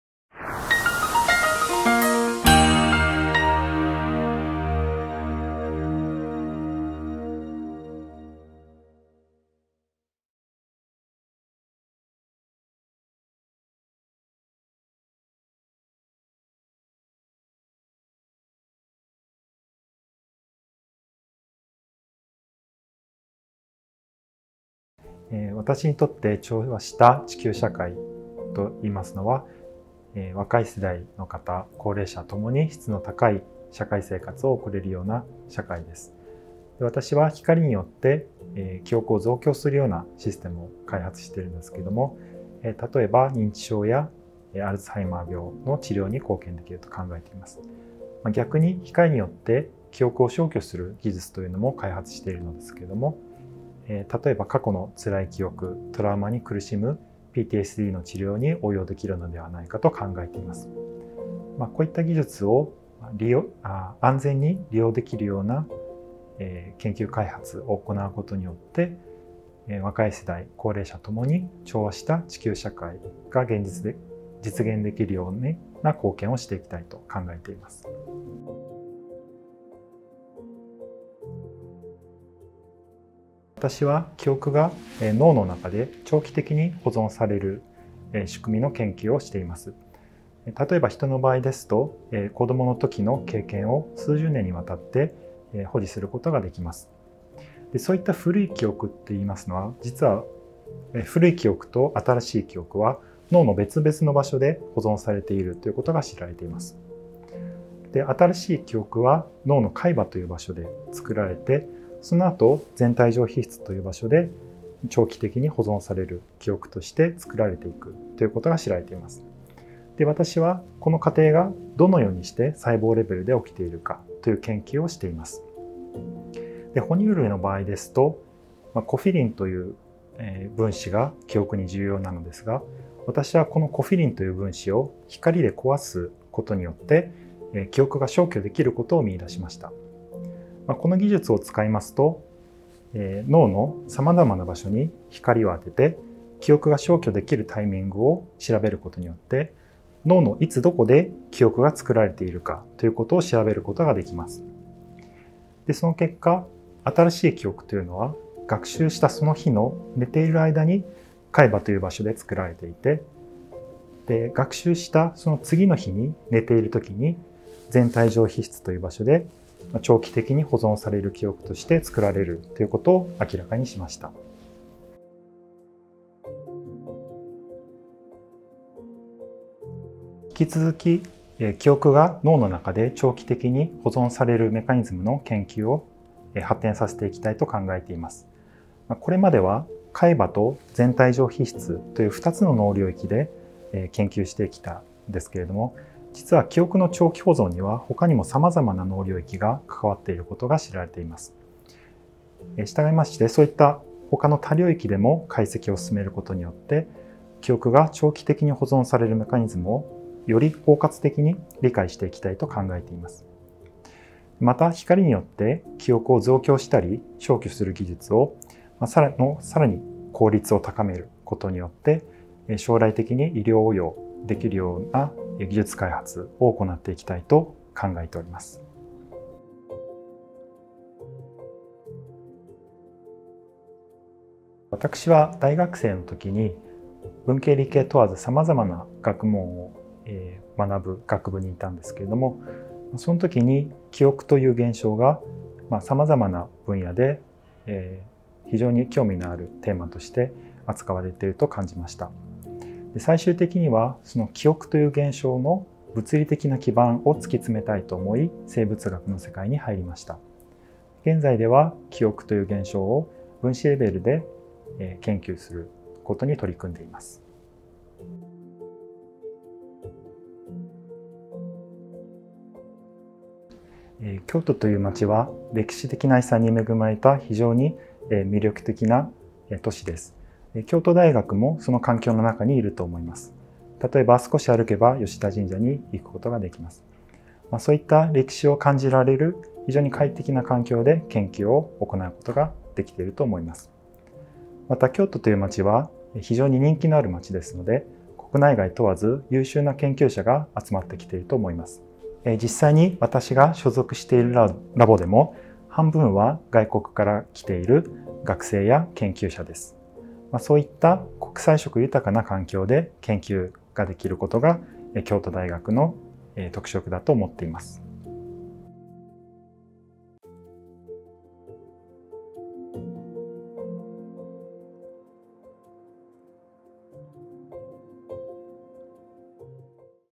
後藤明弘先生：記憶の物理的な実体をつきとめ、人々の生活の質の向上につなげたい。
「くすのき・125」は、京都大学創立125周年記念事業の１つの柱である「研究力強化」の一環として、2020年に始動した斬新かつ大胆な学内ファンドです。
今回は、2022年度に本ファンドに採択された、京都大学医学研究科の後藤明弘 特定准教授にお話を伺いました。本動画は、先生のお話のダイジェストです。
詳しいインタビューの内容は記事にまとめていますので、下記リンクからご覧ください。

※所属・役職・内容は動画制作当時のものです。